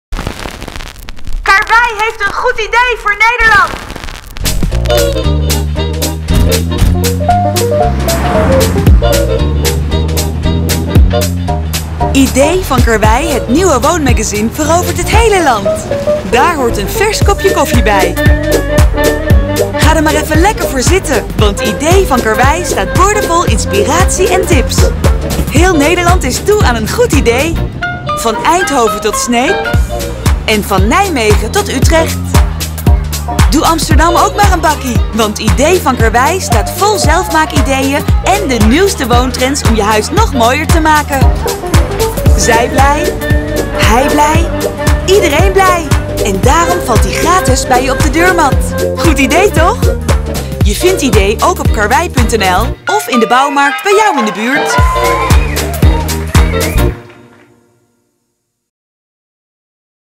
Idee van KARWEI magazine | Een goed idee voor Nederland
Introductie van het nieuwe en gratis inspiratiemagazine Idee van KARWEI. Op de deurmat, op te halen in de bouwmarkt en te lezen op karwei.nl. Ontwerp en realisatie magazine: Dedato ontwerpers en architecten Promotie video: Miriam Bolten voor Giesbers AV Media.